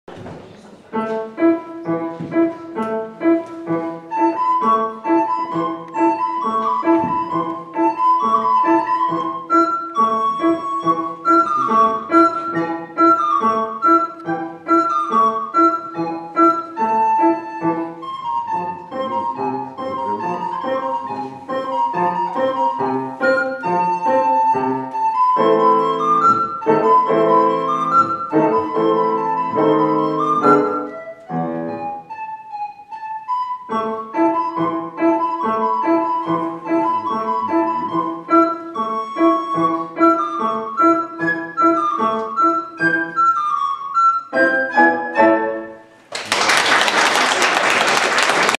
Никандрова Анна - выступление 2 на IV конкурсе блокфлейтистов.
Никандрова Анна-ДМШ им. П.И. Юргенсона преподаватель Оболонская И. А.-концертмейстер Оболонская И. А.